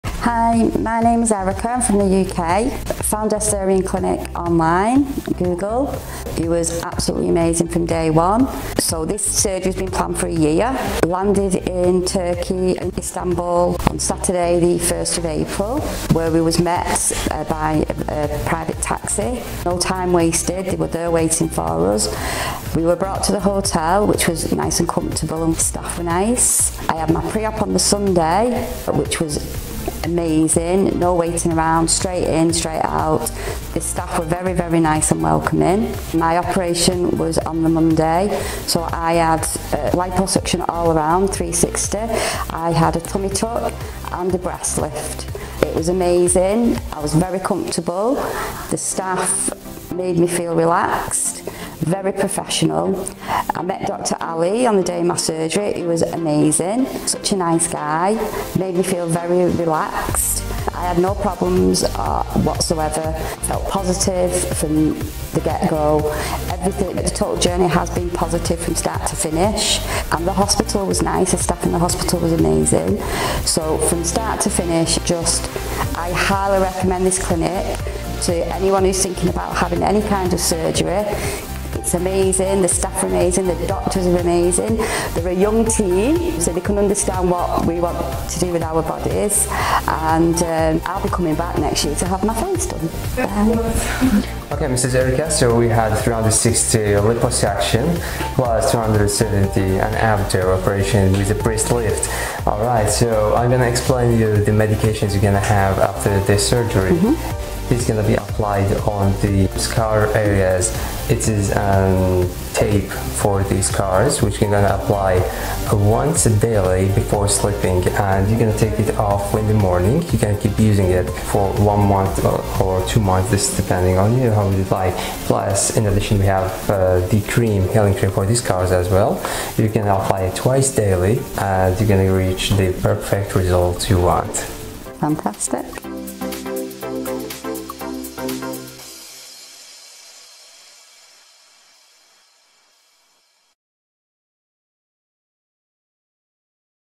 Erica's Breast Lift & Tummy Tuck Surgery Journey in Turkey | Estherian Clinic
Join Erica from the UK as she shares her incredible experience with Estherian Clinic in Istanbul, Turkey. From the moment she landed, Erica was welcomed with exceptional service, including a private taxi transfer, a comfortable hotel stay, and a seamless pre-op consultation. 🏨✈️

Estherian Clinic
Istanbul, Turkey